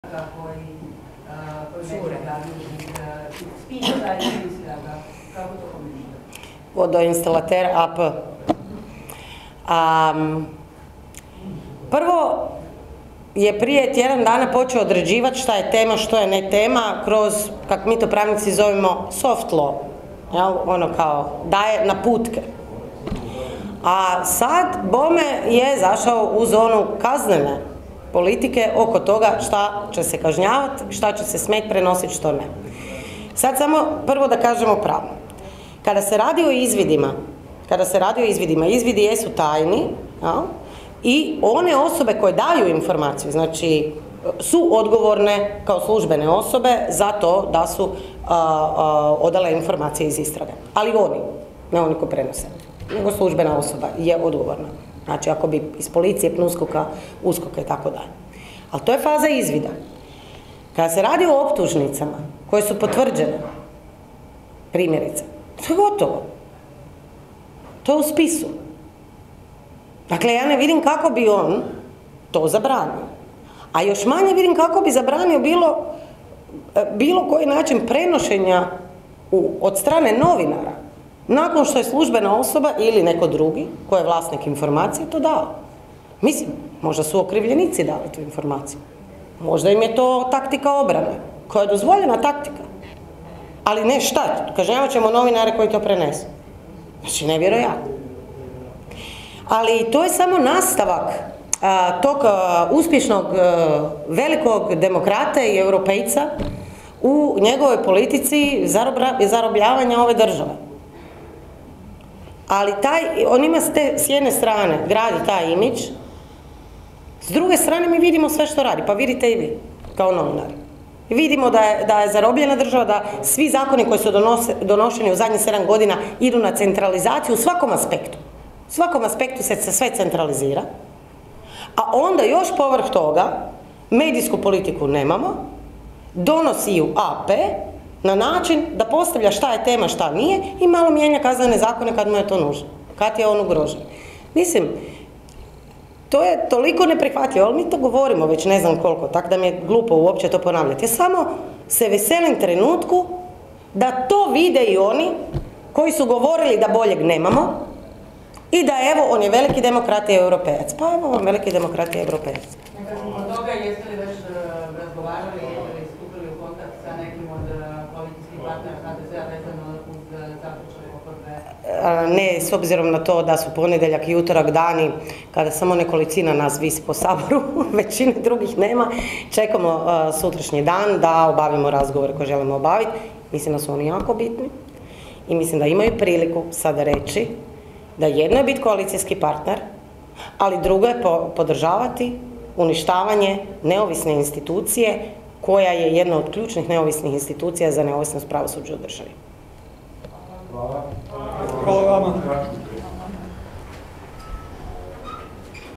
Benčić o Plenkoviću: "AP, veliki demokrata i veliki Europejac"
U Hrvatskom saboru održana je konferencija za medije Kluba zastupnika lijevo zelenog bloka na temu  "Osvrt na novi Prijedlog zakona o obnovi zgrada oštećenih potresom".